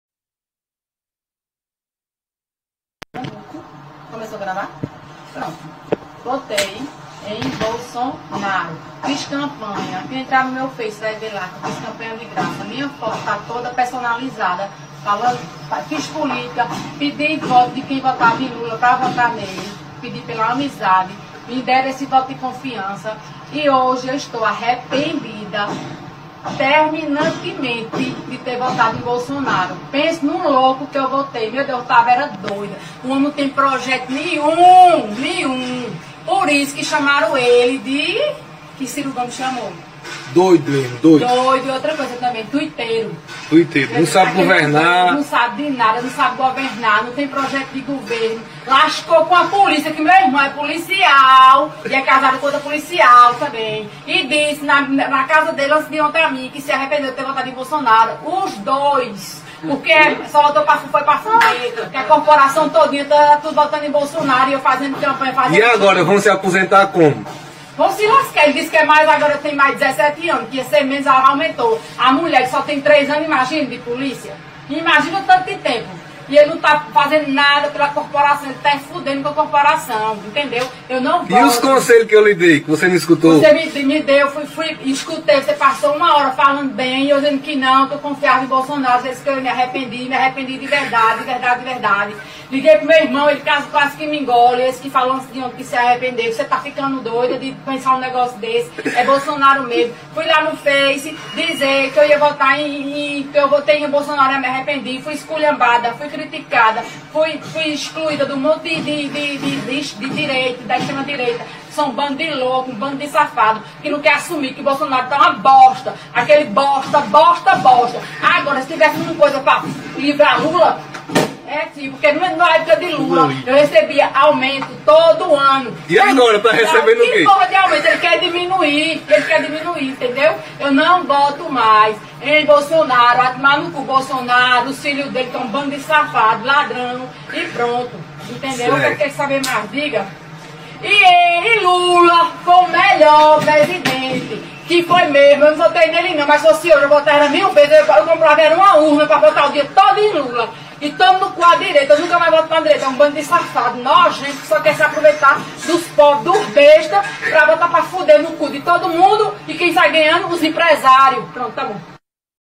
Arrependido? Lula Livre resolve!
Bminion caindo na real. Lula é o Cara!
Não se deixe enganar pela imprensa, a reforma da previdência destrói o Brasil, entrega o nosso futuro na mão dos bancos brasileiros, os piores do mundo.